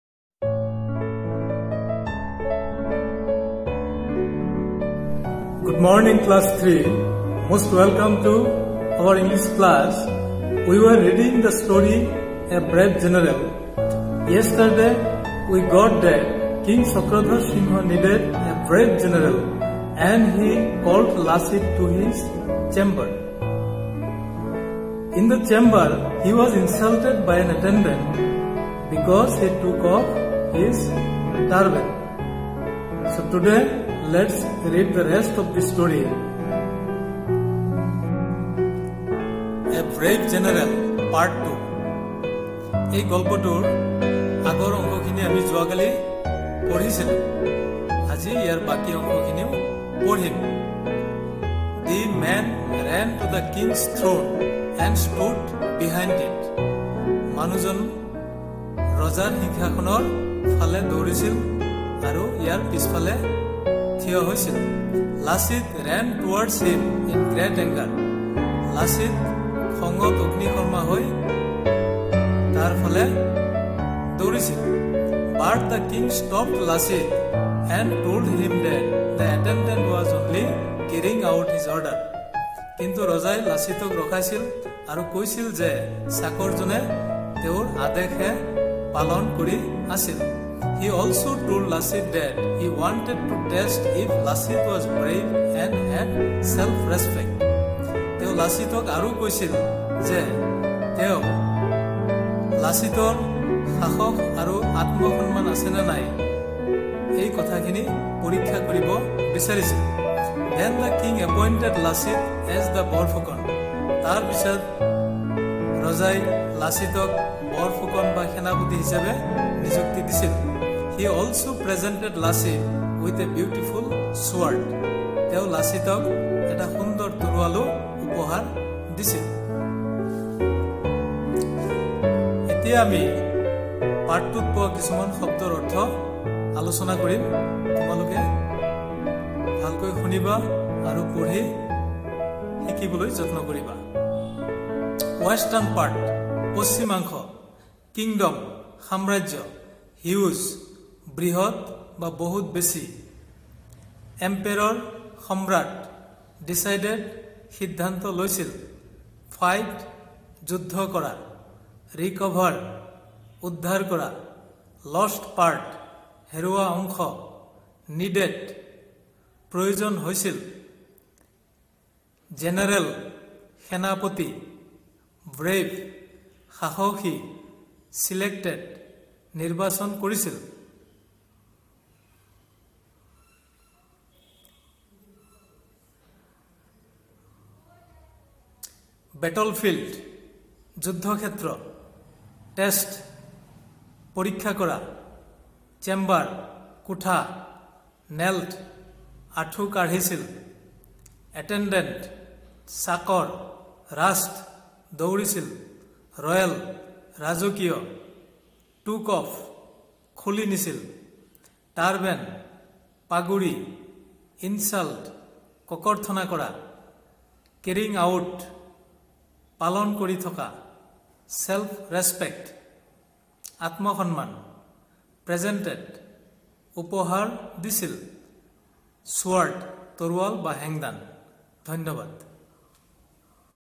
English, class-III, lesson-3 (A brave general), part-2
explaination rest of the lesson and word meanings.